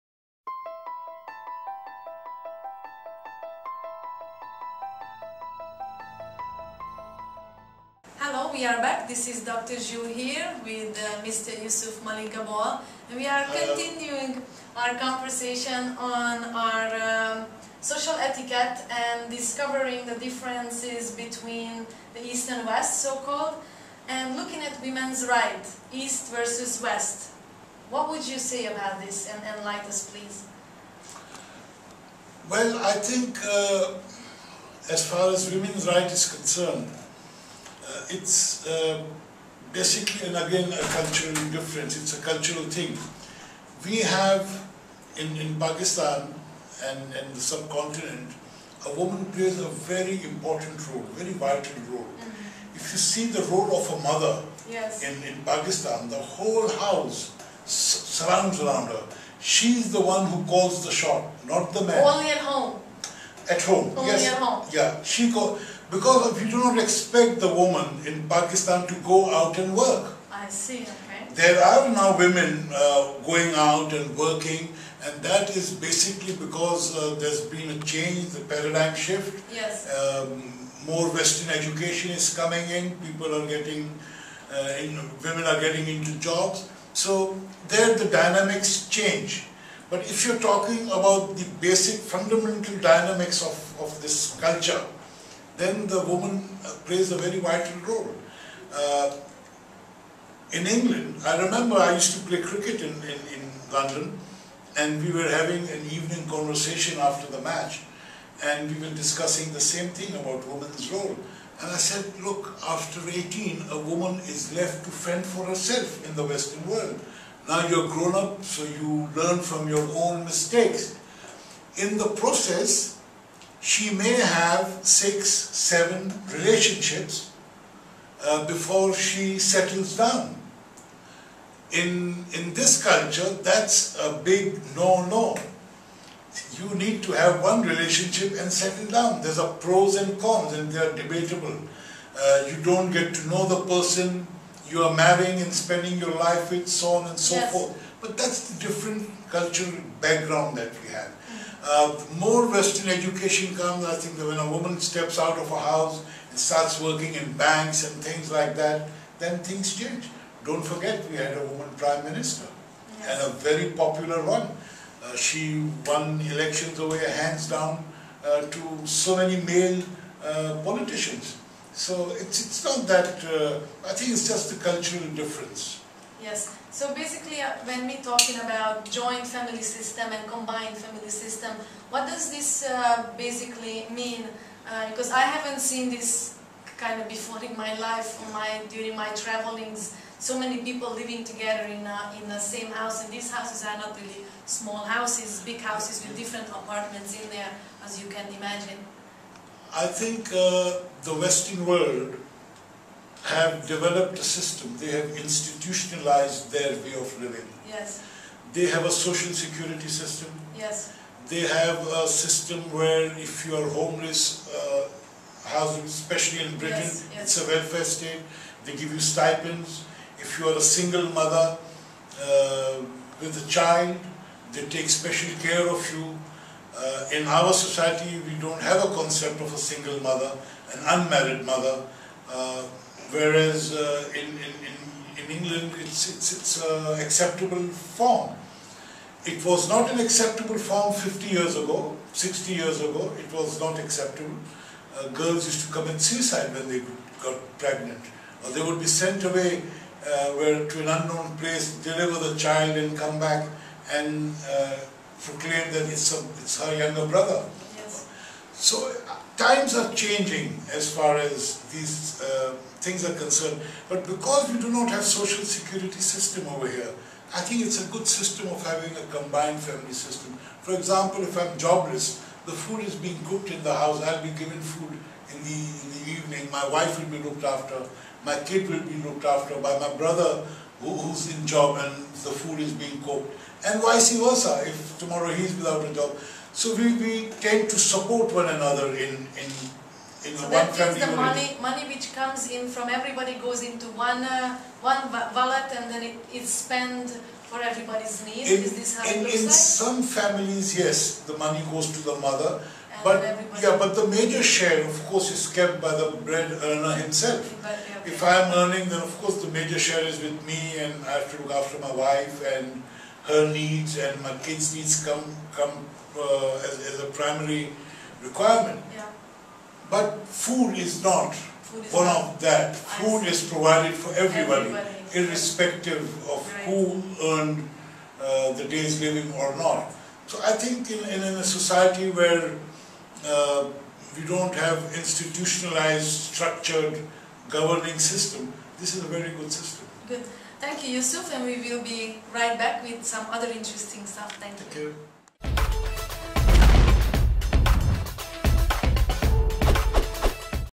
Cultural Differences - Who is the boss at home in the East?
Womens rights, East vs West. What does it mean living in a joint family system. Dr. Zsu and Yusuf Malik Gabol on cultural differences in cultural etiquette to bring understanding to the west about women in the East Pakistan